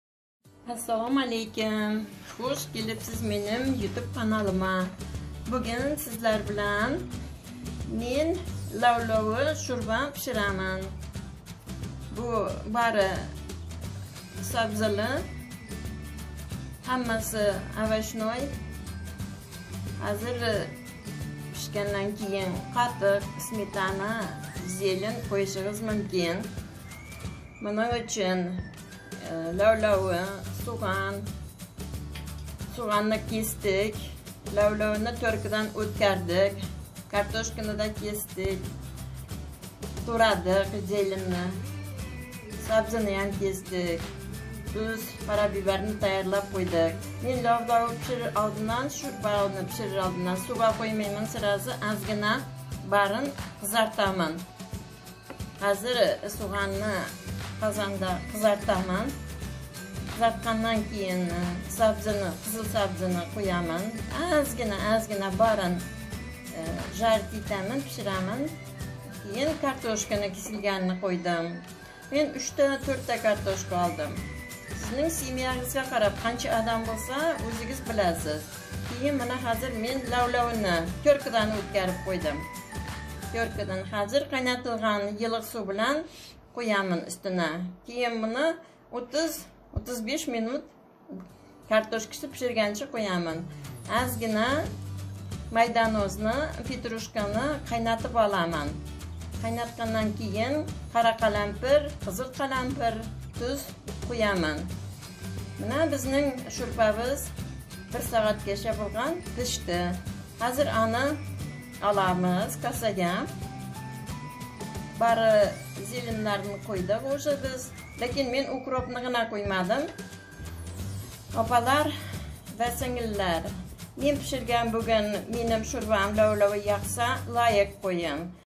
Свёкольный суп на быструю руку.-Lavlagi sho'rva
Свёкольный суп на быструю руку.
2 свела тёртой через тёрку.
2 моркови.3-4 картошки небольшой.
Специй:чёрный .красный перец и соль по вкусу.
Зелень: я положила петрушку и укроп,А вы положите какую вы любите.это всё по вкусу.